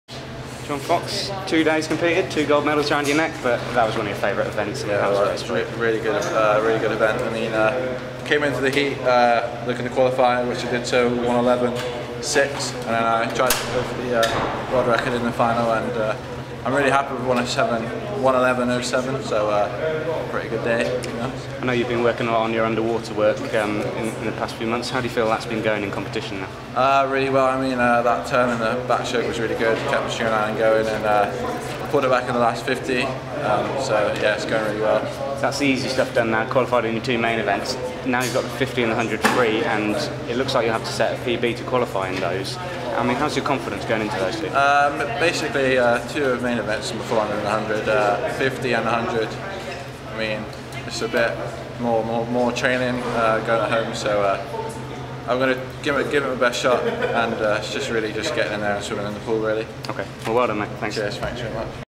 Jonathan Fox defends his MC 100m Backstroke title at British Champs 2012
Jonathan Fox defends his MC 100m Backstroke title at the British Gas Swimming Championships 2012 in London Aquatics Centre.